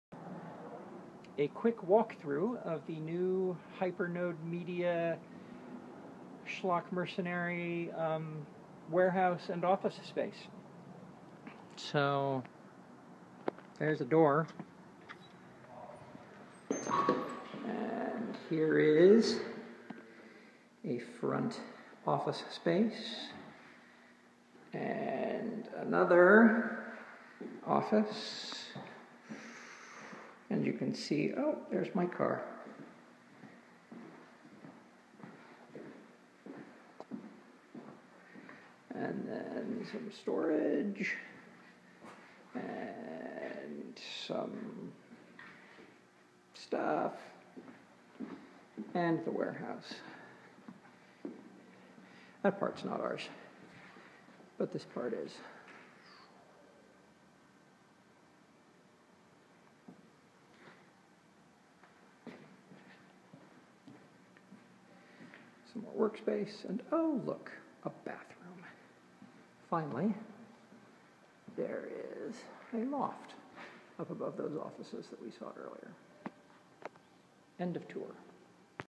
The Hypernode Media/Schlock Mercenary Warehouse Tour
We just signed the lease on this space. Here, then, is a motion-sickness-inducing walkthrough. (Be grateful I did not aim the camera into the toilet -- that would have induced different kinds of sickness.)